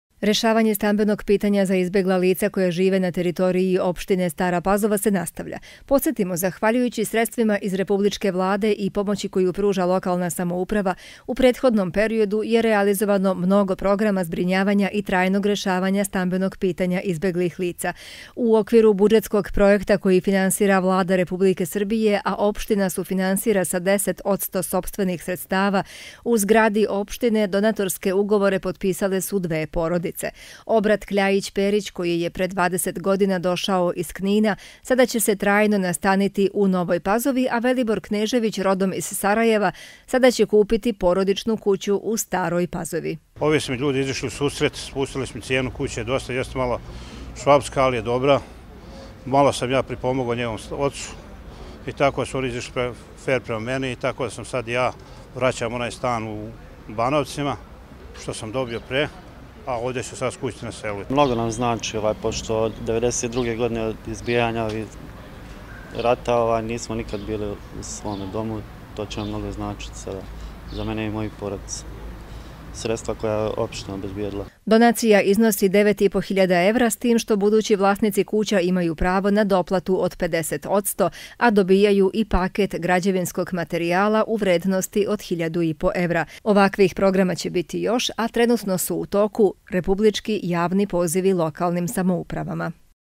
Za još dve izbegle porodice rešeno stambeno pitanje
Za dve izbegle porodice u staropazovačkoj opštini trajno rešeno stambeno pitanje otkupom kuća sa okućnicom.  90 odsto sredstava je iz budžeta Vlade Republike Srbije a 10 odsto finansira Opština Stara Pazova, koje je do sad pomoglo već brojnim izbeglim, progranin I raseljenim licima.